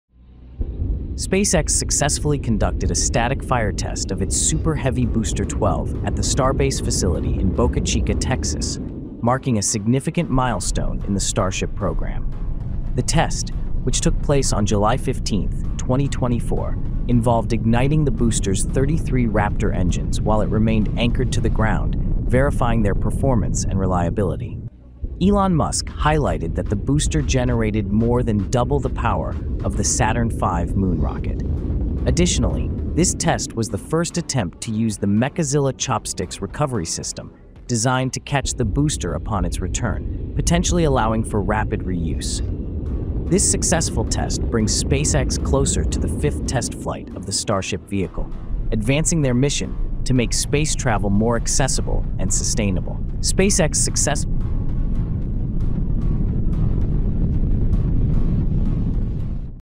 SpaceX Static Fire Test of Super Heavy Booster 12
SpaceX successfully conducted a static fire test of its Super Heavy Booster 12 at the Starbase facility in Boca Chica, Texas, marking a significant milestone in the Starship program. The test, which took place on July 15, 2024, involved igniting the booster’s 33 Raptor engines while it remained anchored to the ground, verifying their performance and reliability. Elon Musk highlighted that the booster generated more than double the power of the Saturn V moon rocket. Additionally, this test was the first attempt to use the Mechazilla Chopsticks recovery system, designed to catch the booster upon its return, potentially allowing for rapid reuse. This successful test brings SpaceX closer to the fifth test flight of the Starship vehicle, advancing their mission to make space travel more accessible and sustainable.